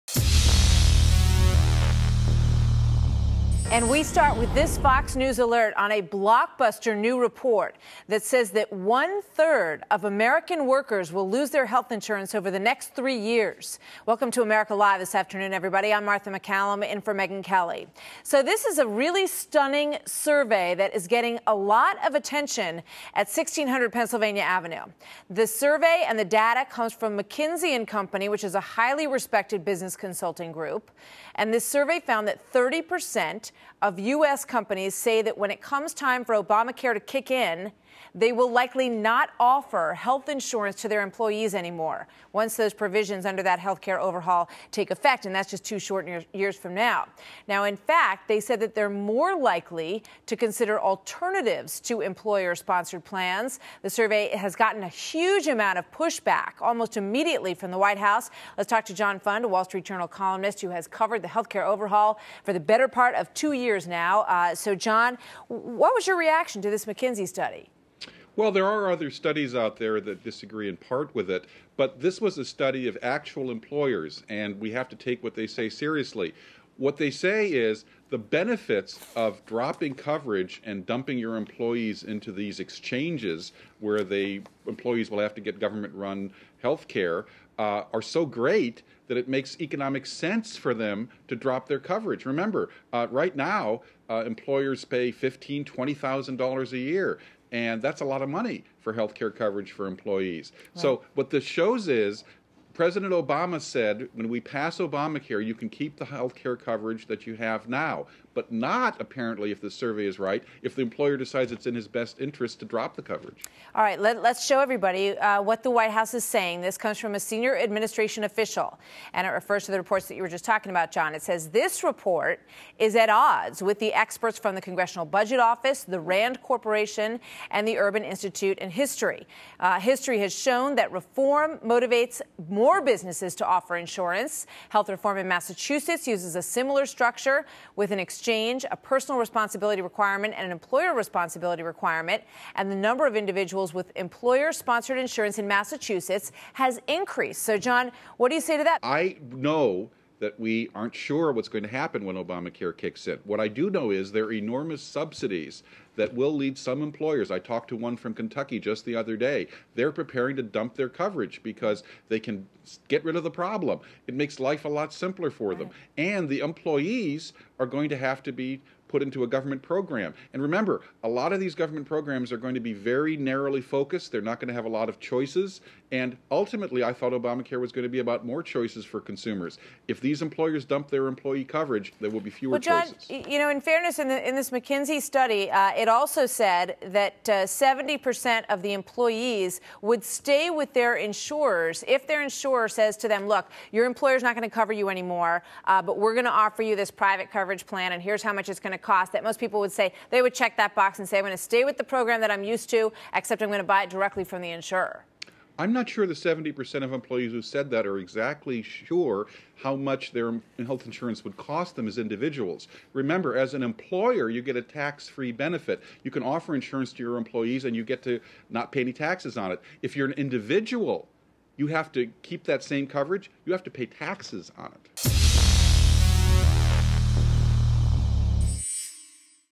Survey: 30% of American Employers to Stop Offering Health Care Coverage When Obamacare Kicks In
According to a new report by McKinsey, 30% of American companies say that when it comes time for Obamacare to kick in, they will likely stop offering health care for their employees.  The White House disputes these findings, but Wall Street Journal Columnist John Fund says, "This was a study of actual employers, and we have to take what they say seriously."  These companies are saying that the benefits of dropping coverage are so great that it makes economic sense for them to drop their coverage.